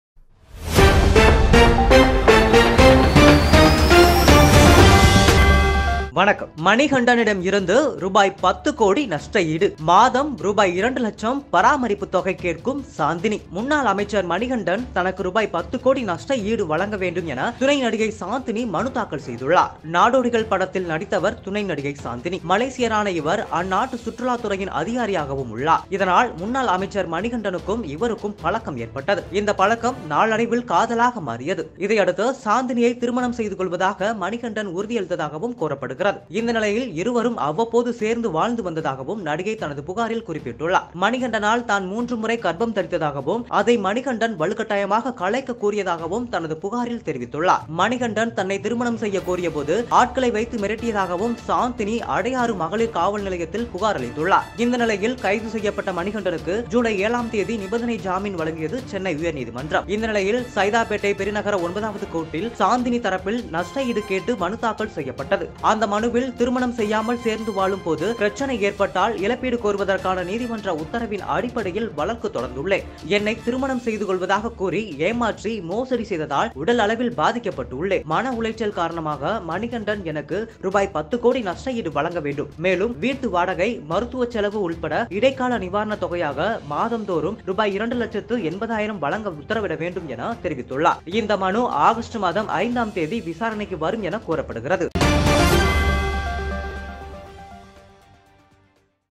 அமைச்சர் மணிகண்டன் ரூ 10 கோடி நஷ்ட ஈடு வழங்க வேண்டும் என நடிகை சாந்தினி மனுதாக்கல்!
முன்னாள் அமைச்சர் மணிகண்டன் தனக்கு ரூ 10 கோடி நஷ்ட ஈடு வழங்க வேண்டும் என துணை நடிகை சாந்தினி மனுதாக்கல் செய்துள்ளார்.